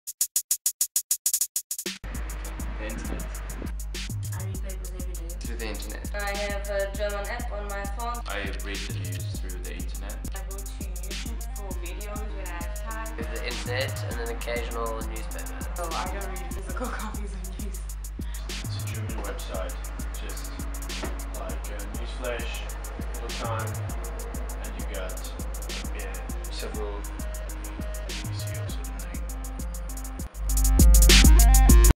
INTERNET! INTERNET! INTERNET!
Students in Stellenbosch state how they consume news. The most popular way? The internet!